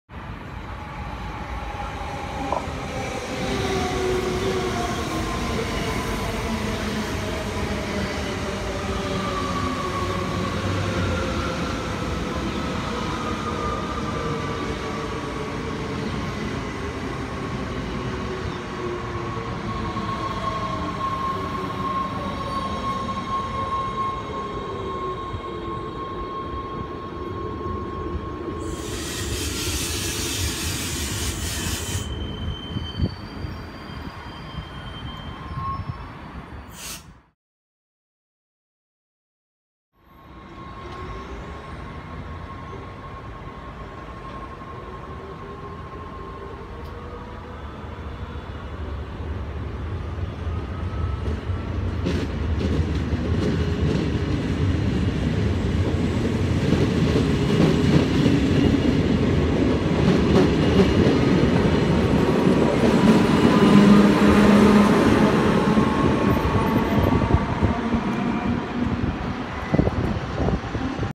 DDM 7202 en loc 1778 halteren op station Rotterdam Noord!
Heel zeldzaam op station RTD Noord! NS zette vandaag bijna op alle zeldzame plekken DD-AR of DDM in. Dit was op donderdag 5 september 2019.